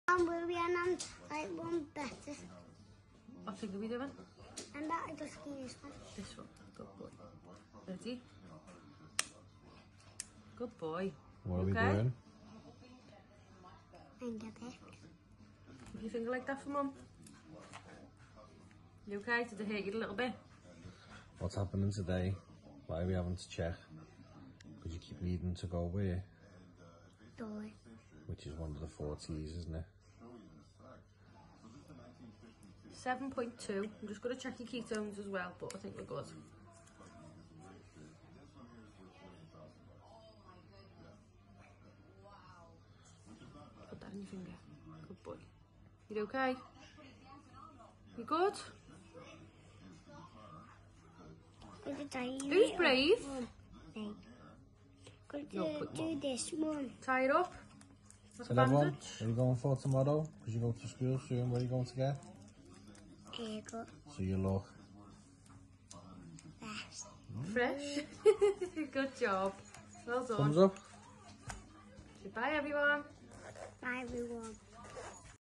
4 year old getting a finger prick ( Louis )
Super proud of my brave little man.